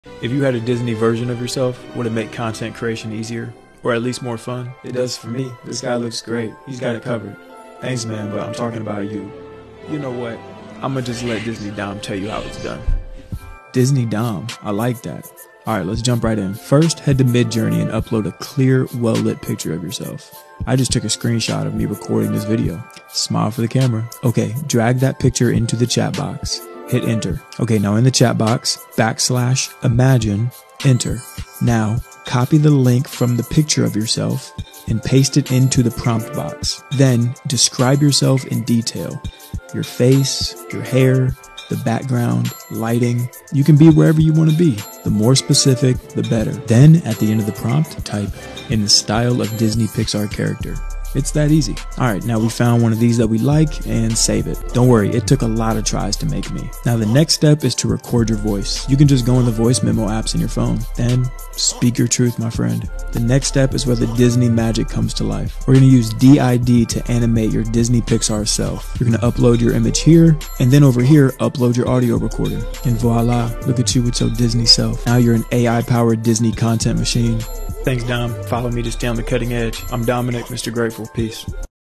Disney-fy Me: Transforming Yourself with AI
Disney-fy Me is an exciting new AI tool that can transform any image of yourself into a Disney-style cartoon character. Using advanced image processing techniques and machine learning algorithms, Disney-fy Me can detect the features of your face and body and convert them into a whimsical, animated style reminiscent of classic Disney films. With Disney-fy Me, you can bring out your inner child and unleash your creativity by seeing yourself in a whole new way. Whether you want to create a personalized avatar, a fun social media profile picture, or just explore the possibilities of AI art, Disney-fy Me is the perfect tool for transforming yourself into a Disney character.